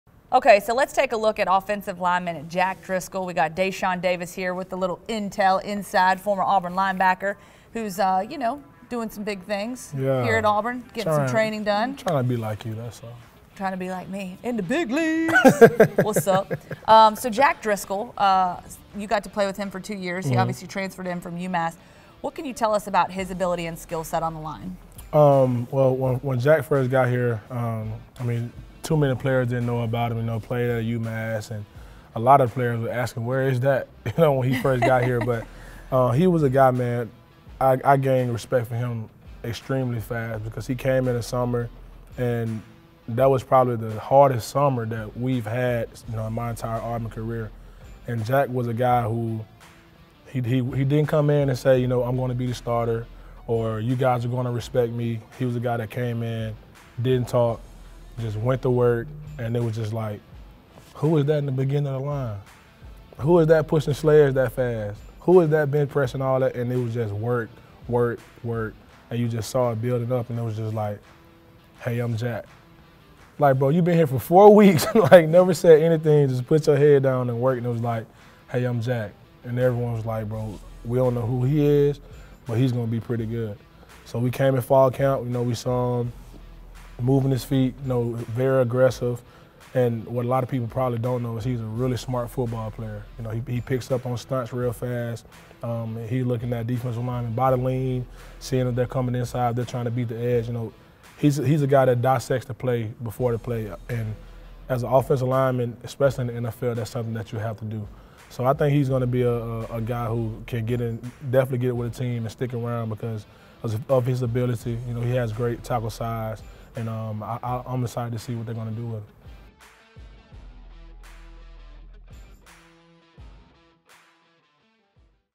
Jack Driscoll NFL Draft profile and analysis
Jack Driscoll made a quick transition from UMass to Auburn, and his former teammate Deshaun Davis believes he could do the same from Auburn to the NFL. (Wesley Sinor / )